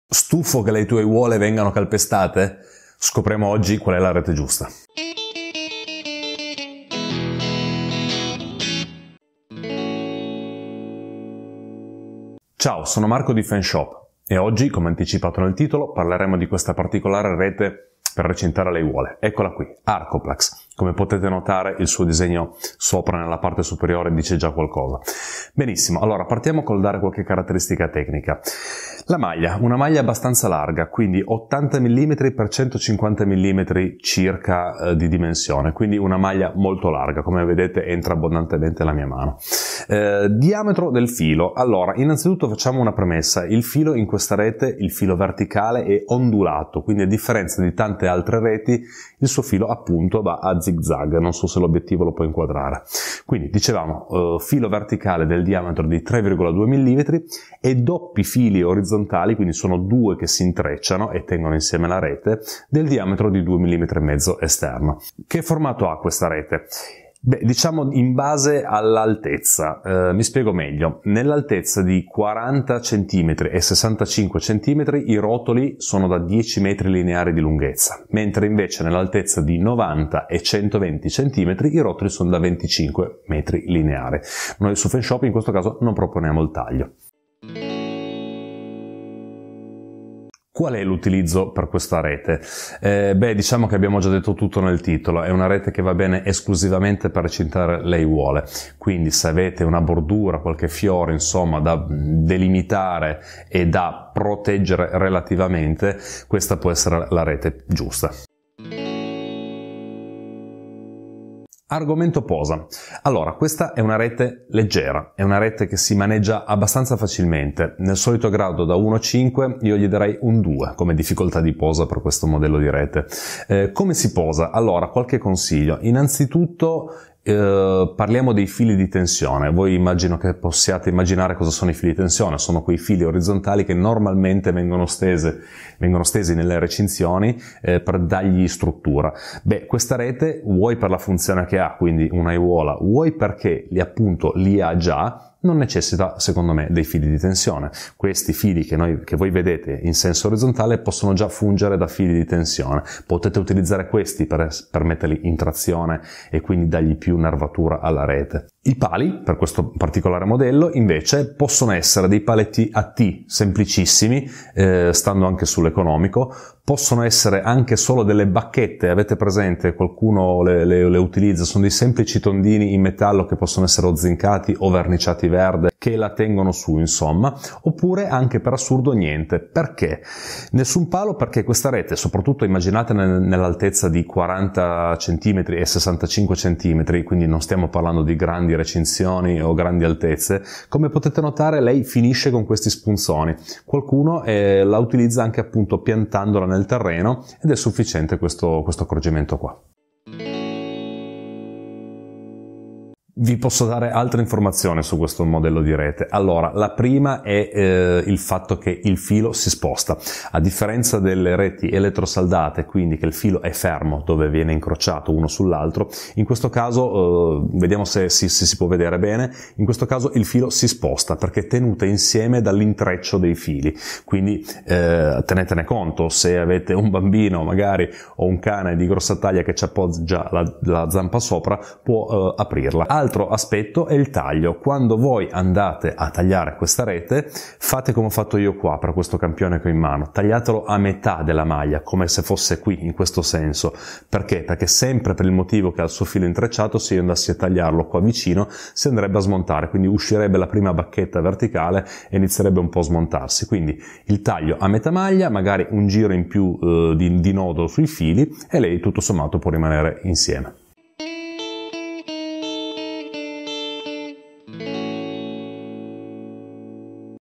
Rete metallica per aiuole. Vediamo le sue caratteristiche
Stufo che le tue aiuole vengano calpestate? Scopriamo oggi qual’è la rete giusta!
Ciao sono Marco di Fenceshop e oggi, come anticipato nel titolo, parleremo di questa particolare rete per recintare aiuole. Arcoplax è il suo nome e come potete notare, il suo disegno nella parte superiore dice già qualcosa.

Benissimo! Allora partiamo col dare qualche caratteristica tecnica: la maglia è una maglia abbastanza larga, 80x150 mm circa di dimensione. Come vedete entra abbondantemente la mia mano. Facciamo una premessa, il filo in questa é ondulato, quindi a differenza di tante altre reti il suo filo appunto va a zig zag. Il filo verticale è del diametro di 3,2 mm e i doppi fili orizzontali sono del diametro di 2,2 mm che si intrecciano. Parlando di altezze, i rotoli di 40 cm e 65 cm di altezza, sono lunghi 10 metri, mentre invece nelle altezze di 90 e 120 cm i rotoli sono lunghi 25 metri. Noi su Fenceshop in questo caso non proponiamo il taglio al metro.

Qual’è l'utilizzo per questa rete? Diciamo che abbiamo già detto tutto nel titolo! E’ una rete che va bene esclusivamente per le aiuole, quindi se avete una bordura con qualche fiore da delimitare e da proteggere va più che bene ma non andrei oltre a questo utilizzo.

Per quanto riguarda la posa in opera, questa è una rete leggera, è una rete che si maneggia abbastanza facilmente e nel solito grado da 1 a 5 io gli darei un livello 2 come difficoltà di posa. Qualche consiglio di posa: Innanzitutto parliamo dei fili di tensione che sono quei fili orizzontali che normalmente vengono stesi nelle recinzioni per dargli struttura. Questa rete, per la funzione che ha (quindi un aiuola) ha già dei fili orizzontali che posso già servire già da fili di tensione. Potete utilizzare questi per metterla in trazione e quindi dargli più robustezza.

Quali pali usare? Essendo una rete leggera e considerando che serve come bordura, possono essere dei paletti a T semplicissimi che sono anche più economici. Vanno bene anche solo delle bacchette! Avete presente dei semplici tondini in metallo che possono essere zincati o verniciati verdi che la tengono su? Per assurdo potete anche non mettere pali perché questa rete (soprattutto immaginata nella altezza di 40 cm e 65 cm quindi non stiamo parlando di grandi Recinzioni o grandi altezze) come potete notare, finisce con queste punte. Qualcuno la utilizza anche appunto piantandola nel terreno ed è sufficiente questo accorgimento per posarla a terra.

Vi do altre due informazioni su questo modello di rete!
Allora, la prima è il fatto che il filo si sposta! A differenza delle reti elettrosaldate quindi che il filo è fermo dove viene incrociato uno sull'altro, in questo caso, il filo si sposta perché è tenuto insieme dall'intreccio dei fili stessi. Tenetene quindi conto se avete un bambino o un cane di grossa taglia che si appoggia con la zampa sopra.
La seconda informazione è il taglio. Quando tagliate questa rete, fate come ho fatto io in questo video, tagliatelo a metà della maglia sempre per il motivo dei fili intrecciati. Se andassi a tagliarlo vicino al filo verticale, si potrebbe smontare. Uscirebbe quindi la prima bacchetta verticale e potrebbe smontarsi. Quindi taglio a metà maglia e poi un giro in più il filo in modo da creare un nodo per tenerla insieme.